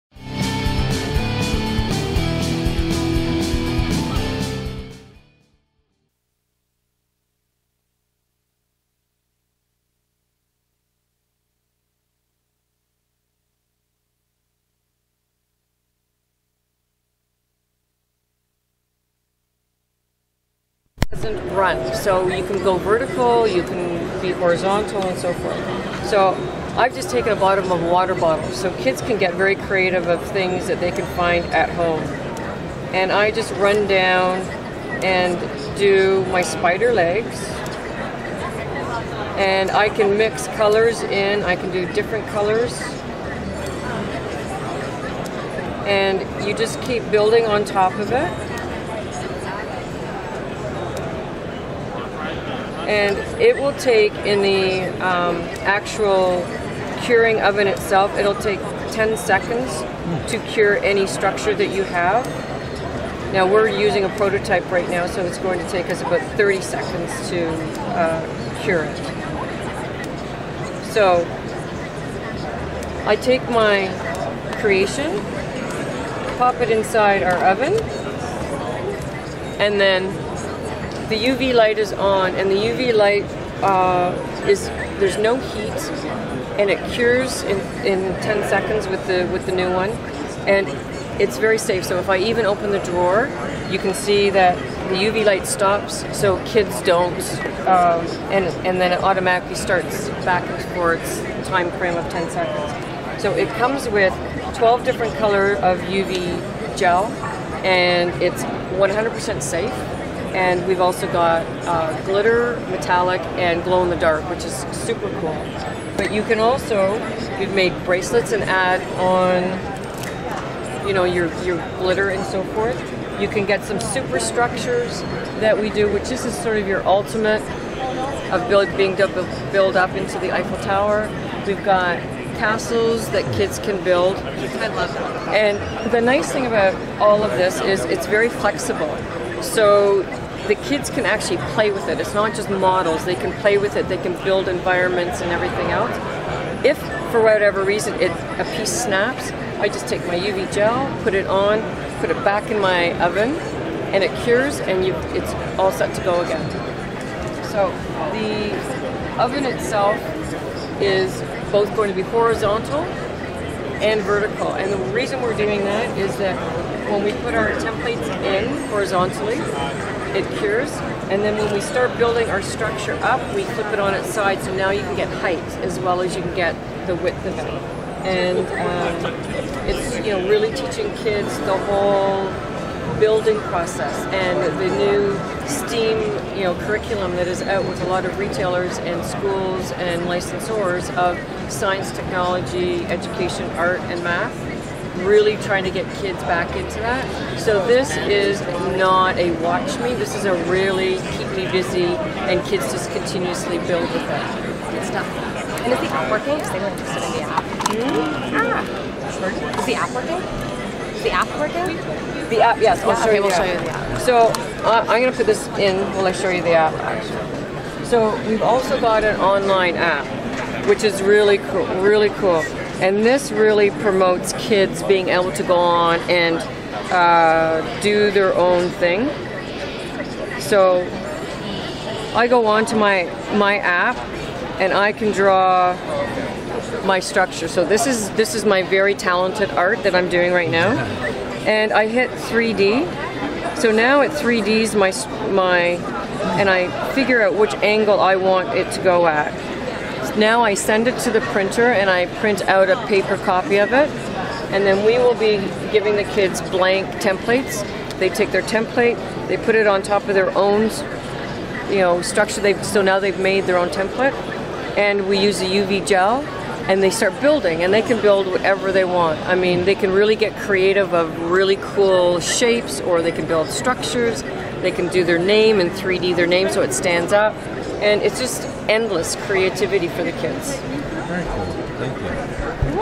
3D Magic Creation Maker
3D Magic Creation Maker as filmed by BSCKids at Toy Fair 2015.

Using a gel that is hardened in UV light the 3D Magic Creation Maker is like a kids 3D printer to build all sorts of cool things.